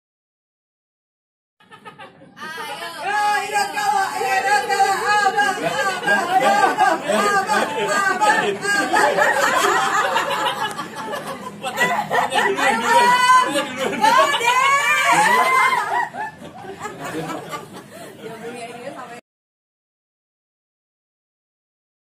M ZOHRI // DAPAT HADIAH PALING SPESIAL DARI KAMI
Video ini di buat sebagai apresiasi atas prestasi M Zohri yg luar biasa.
ternyata ga gampang lari di lintasan.ngos2an n pegel choy.saya sdh buktikan sendiri.
Hebat M Zohri .